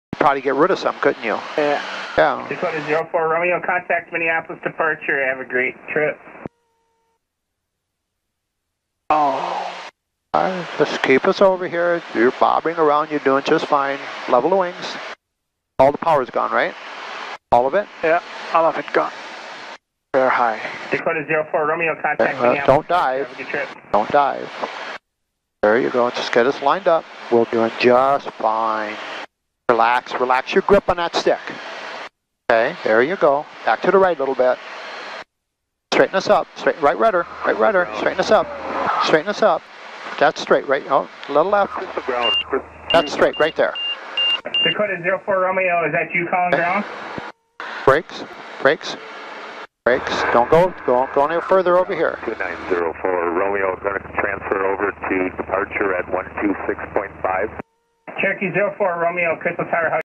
Landing: Instructor asks Student pilot Landing without power
This video from Episode 12, student pilot performed a great landing, while performing Instructor checked with the student pilot whether Student pilot Landing without power.